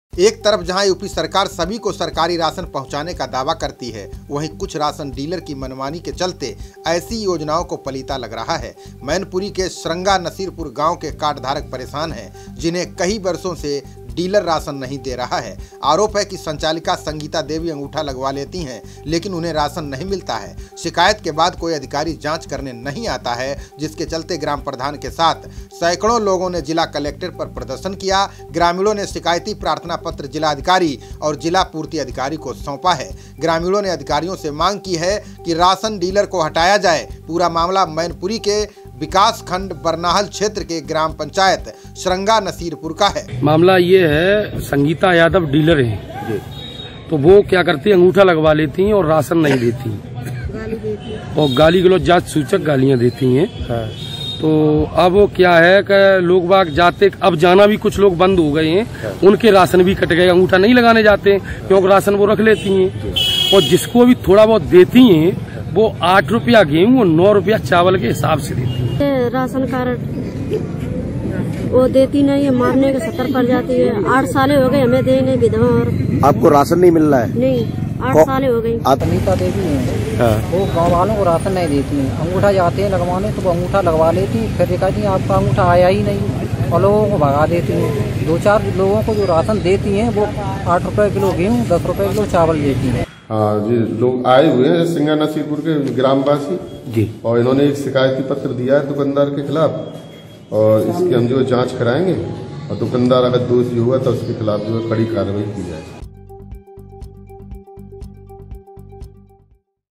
पूरा गांव एकजुट फिर क्यों नहीं हटाया जा रहा राशन डीलर..
राशन डीलर की मनमानी
योजनाओं को लग रहा है पलीता
श्रृंगा नसीरपुर गांव के कार्ड धारक परेशान
कई वर्षों से राशन नहीं दे रहा है डीलर
ग्राम प्रधान के ग्रामीण पहुंचे कलेक्ट्रेट
ग्रामीणोंं ने दिया शिकायती प्रार्थना पत्र